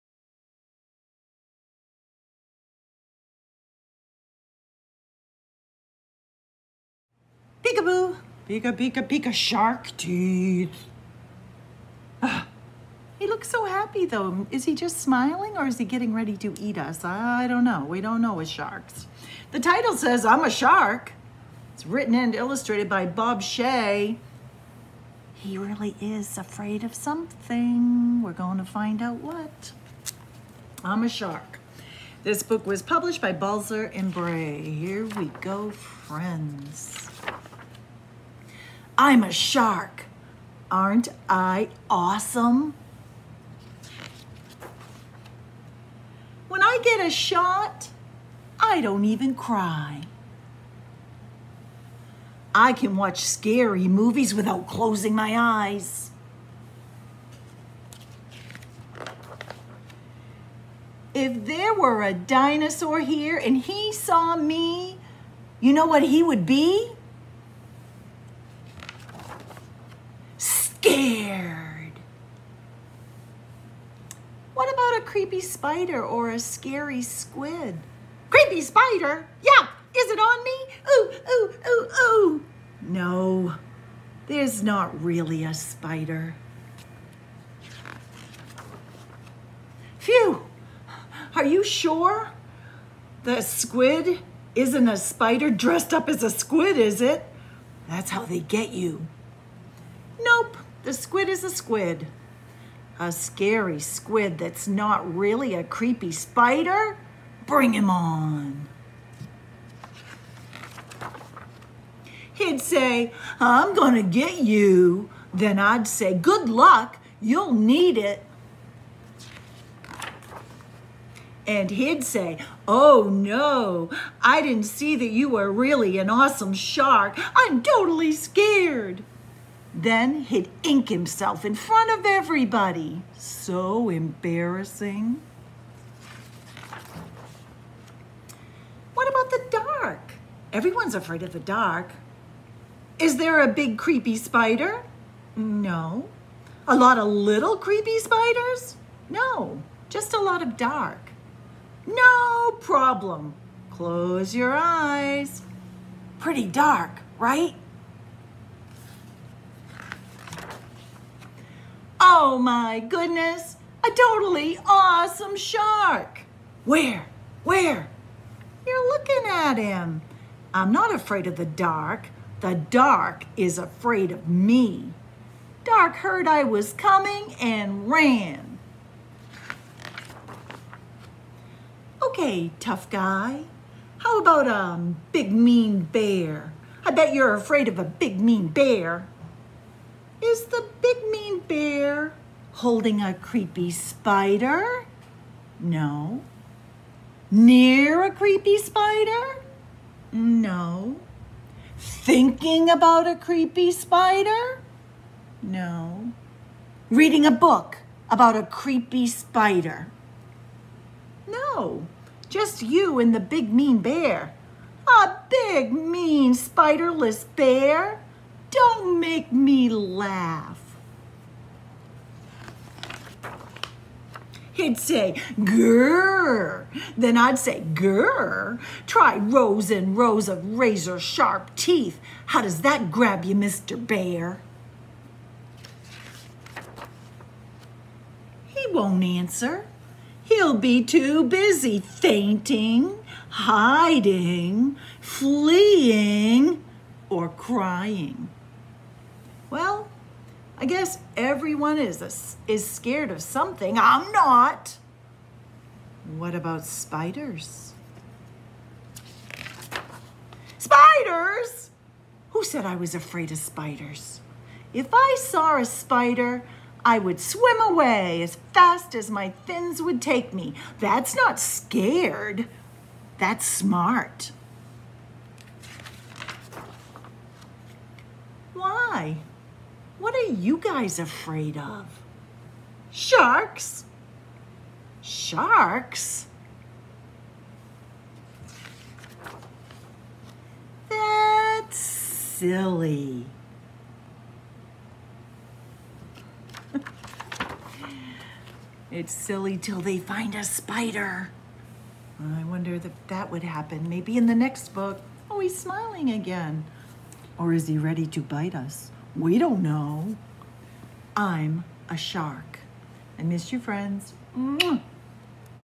Story Time with Miss Cindy
Read aloud picture book, I’m a Shark, written by Bob Shea and published by Balzer and Bray.